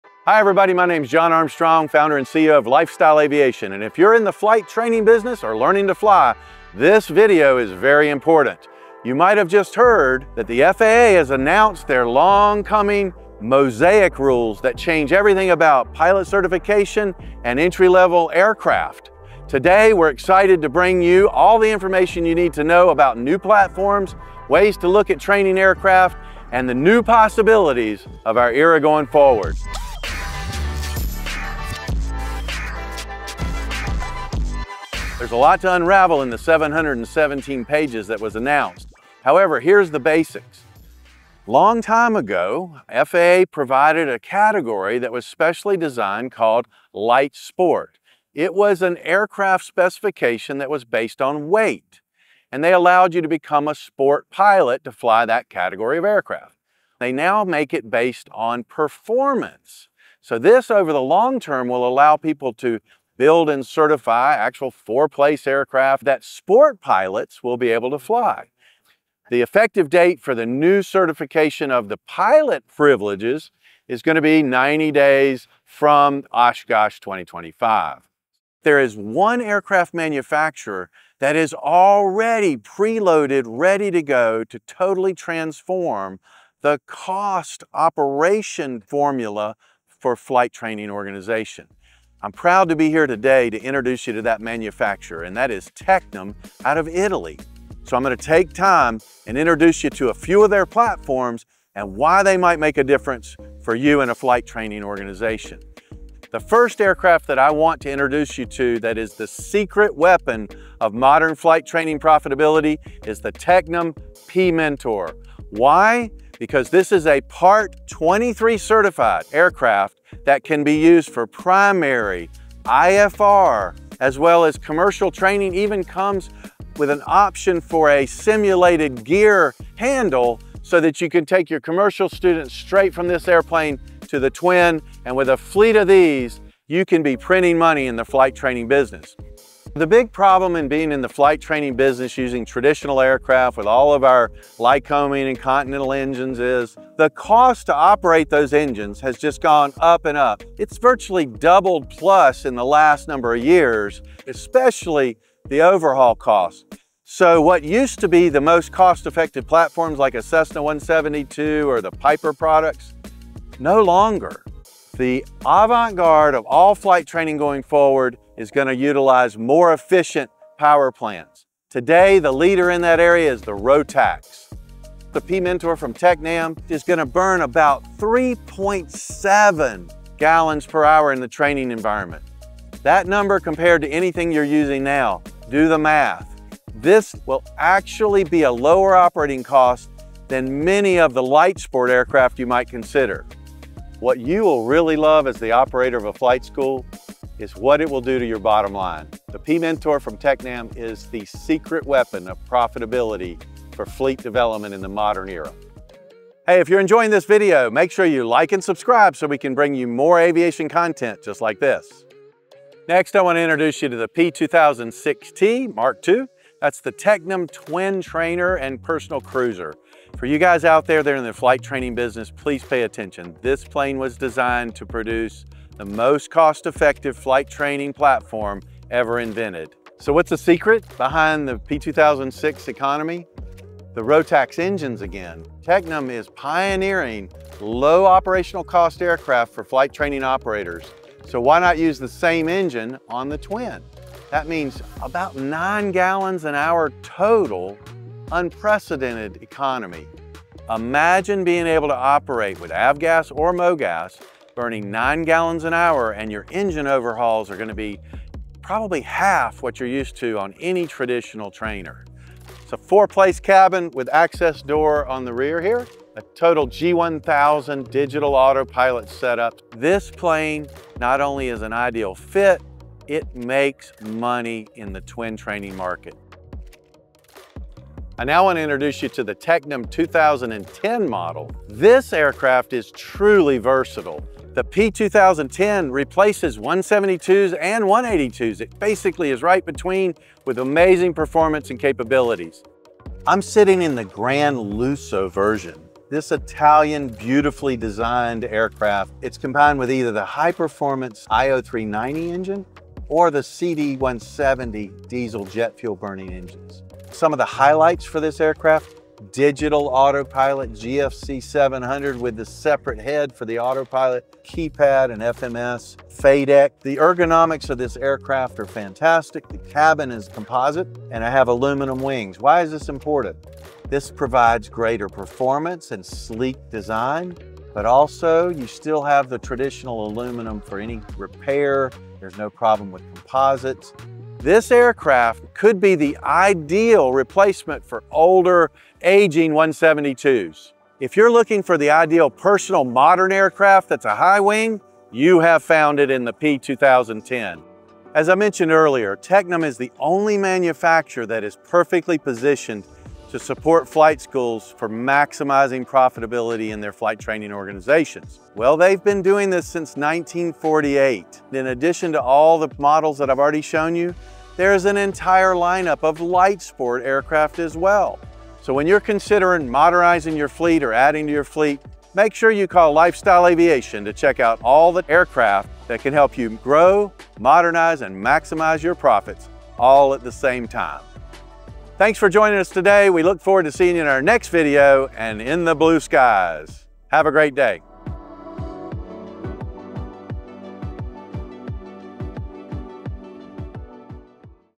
FAA Mosaic Is Here — And Here's Why Tecnam Wins in Flight Training
The FAA’s new Mosaic rules are reshaping the conversation around flight training, and Tecnam is leading the way with modern aircraft designed to lower costs and increase efficiency for flight schools. In this video, John Armstrong, Founder & CEO of LifeStyle Aviation, walks through Tecnam’s most innovative platforms:

P-Mentor – the Part 23 certified trainer that delivers IFR capability and unmatched operating efficiency.
P2006T – a twin-engine trainer that redefines economy with Rotax power.
P2010 – a versatile four-seat high-wing that blends Italian design with modern performance.

Whether you’re a flight school owner looking to improve profitability, a student pilot curious about the next generation of training aircraft, or an aviation enthusiast following Mosaic’s impact, this video shows why Tecnam is uniquely positioned to support the future of flight training.